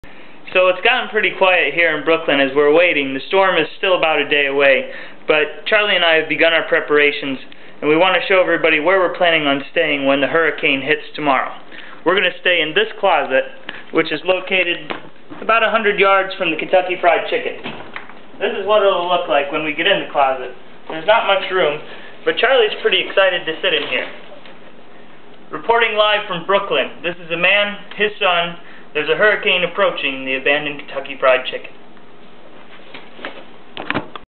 Closet Hideout.MOV
With Hurricane Irene drawing closer to an abandoned Brooklyn KFC, a man and his son start seeking potential safe zones within their apartment.  This video and more are available from the Online Journal of A Man, His Son, a Hurricane, and an Abandoned KFC